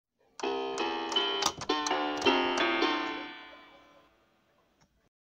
TVO Video Logo (My Version)
Lighting made the TVO Video Logo. Now is my turn.